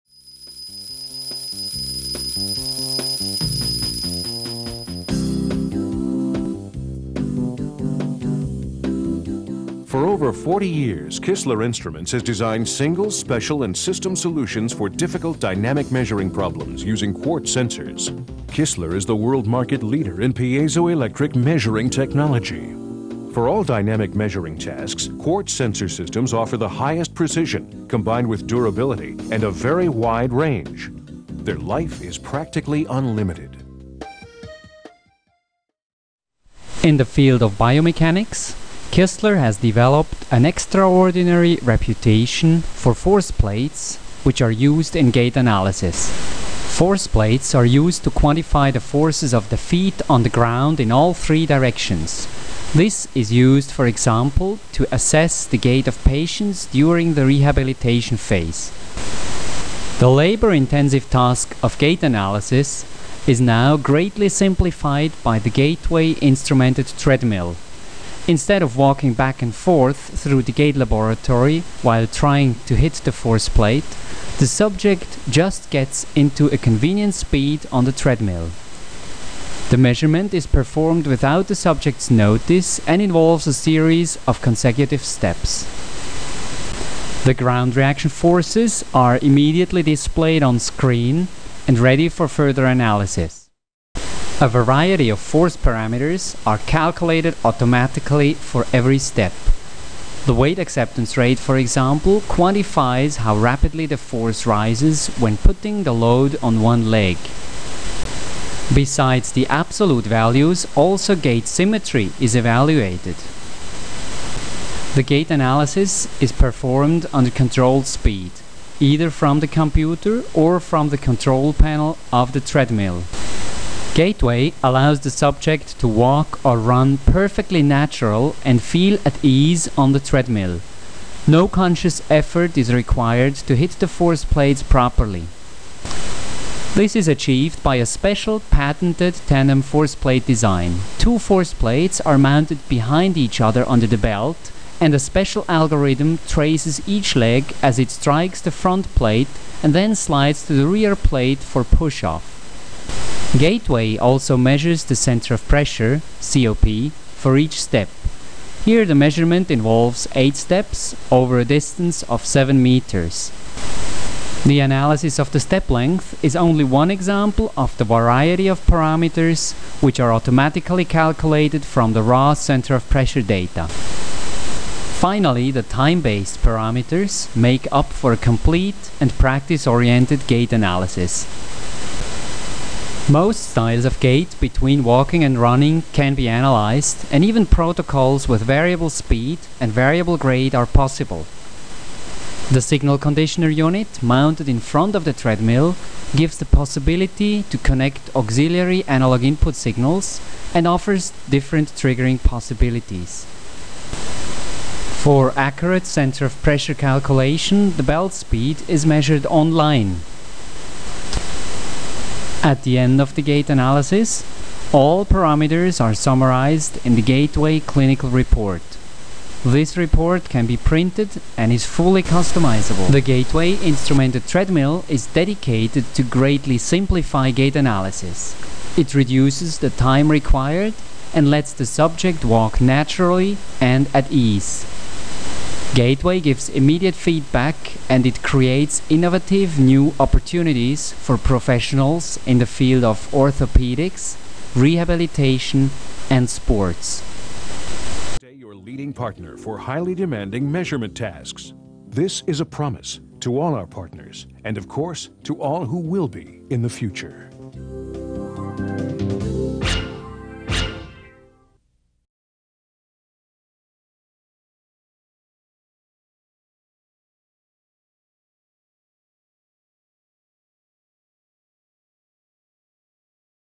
h/p/cosmos KISTLER Gaitway biomechanics treadmill
gaitway is a complete gait analysis system housed in a running machine, developed and manufactured in cooperation of the two companies KISTLER and h/p/cosmos. What makes this treadmill unique is the ability to measure vertical ground reaction forces and center of pressure for complete, consecutive foot strikes during walking and running.

 The instrumented treadmill system has been designed using a patented tandem force plate design and includes a patented algorithm which distinguishes left and right foot-strikes. gaitway simplifies the challenges associated with KISTLER force plate targeting found in conventional walkway systems.